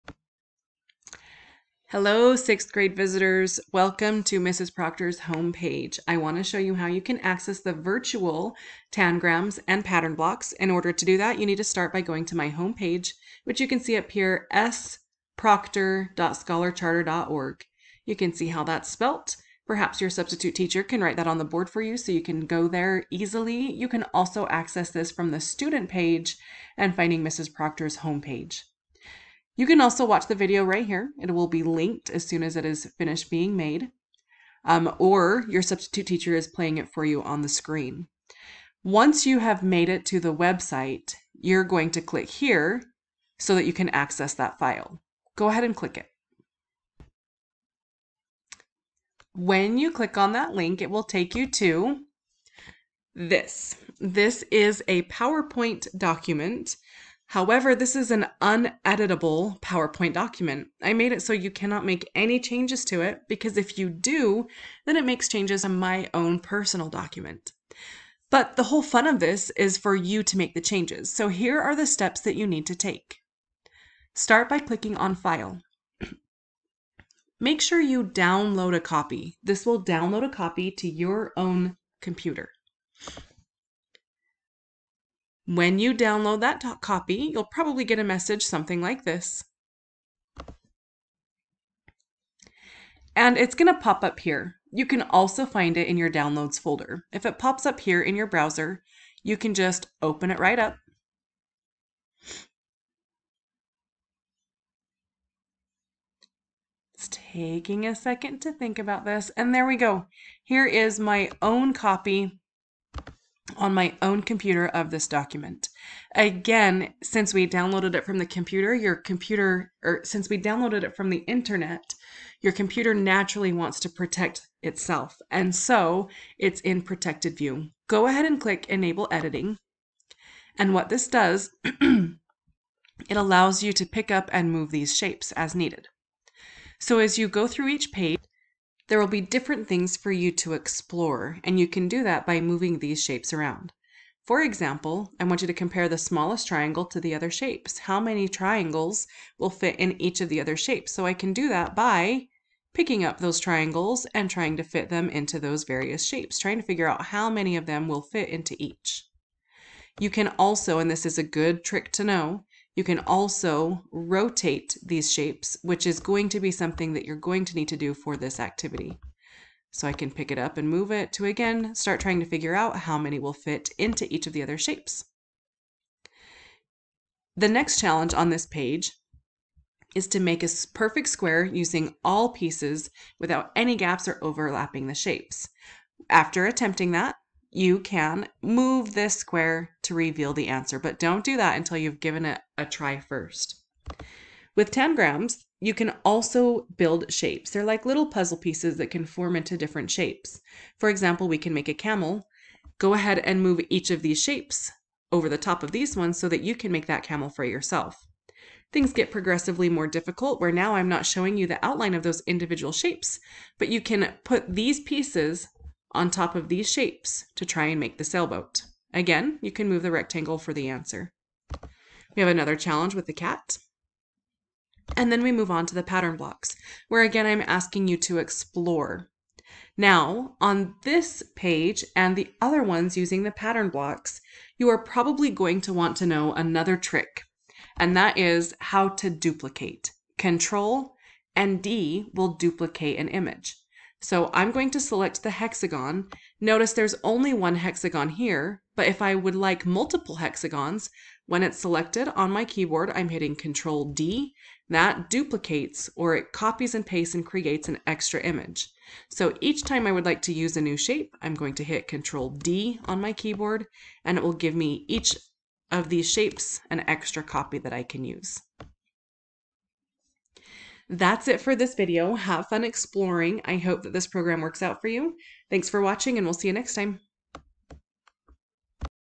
Downloading and Using Virtual Tangrams and Pattern Blocks
Instructions on how to download a copy of the virtual tangrams and pattern blocks Power Point for virtual exploration.